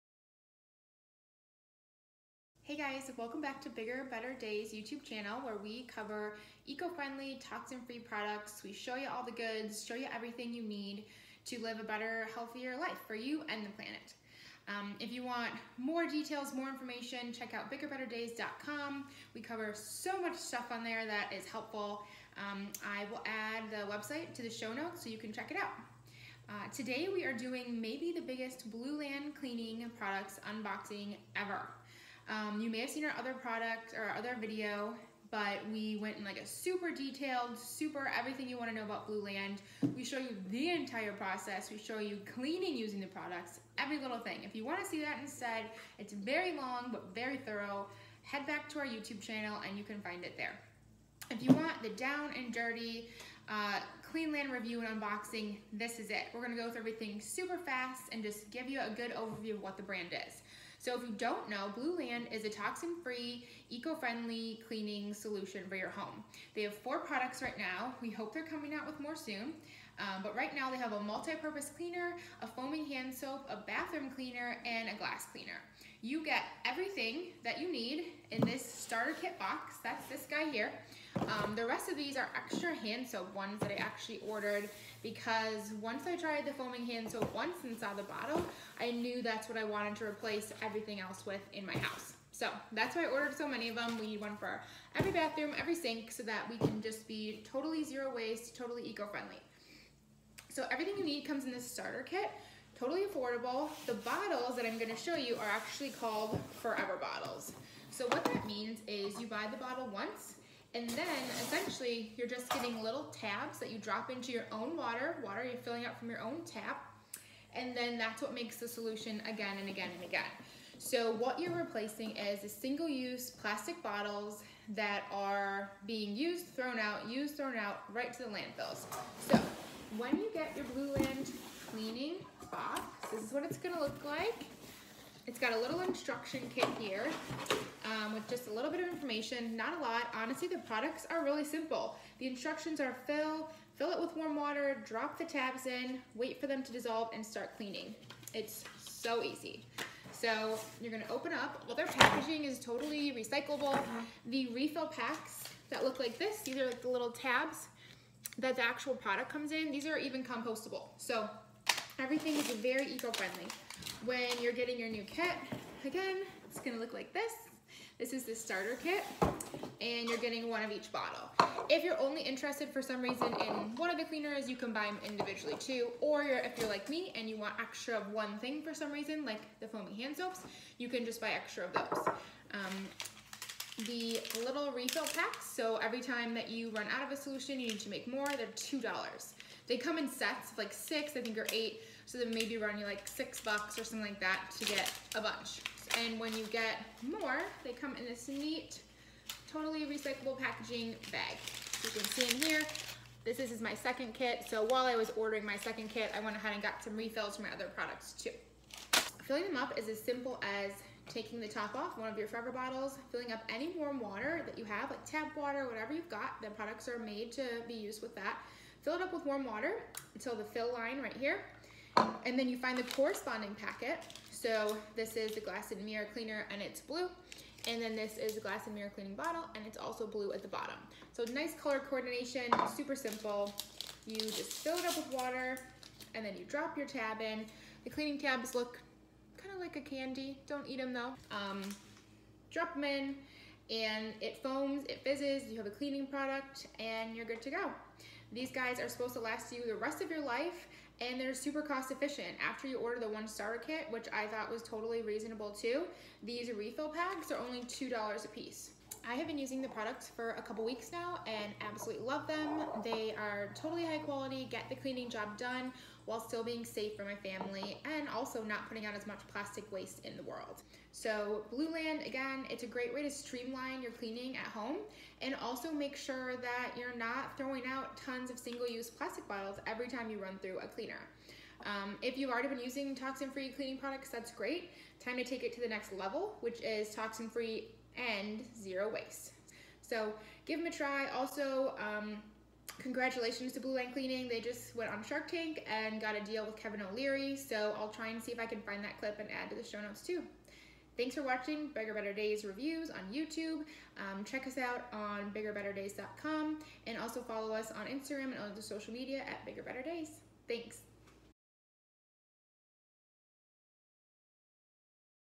Biggest Blueland Cleaning Unboxing Ever! Updated Blueland Cleaning Review
The biggest Blueland cleaning unboxing yet! See our updated Blueland cleaning video.

Blueland cleaning ultimate unboxing and product review video. See exactly how Blueland cleaning toxin free and zero waste cleaning products can help streamline your toxin free home, and produce less waste!

Check out our original complete Blueland cleaning products review with cleaning demos

For more information on toxin free and eco friendly living, check out biggerbetterdays.com!